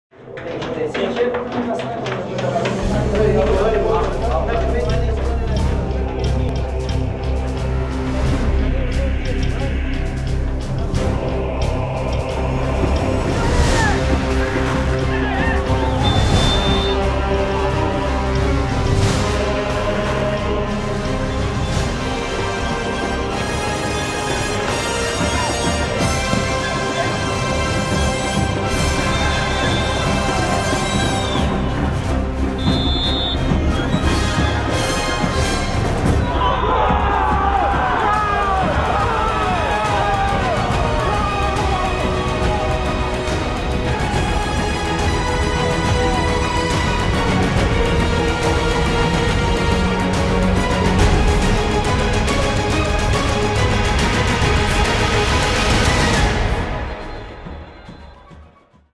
[PRIMERA] ⚽ GOLAAAAAZO ⚽
Atlas batallaba en todos los sectores del campo de juego. Partido más que difícil y una genialidad de Julio Gauna abría el marcador para el equipodelcontinente .

Segmento presentado por Grupo Provincia -Banco Provincia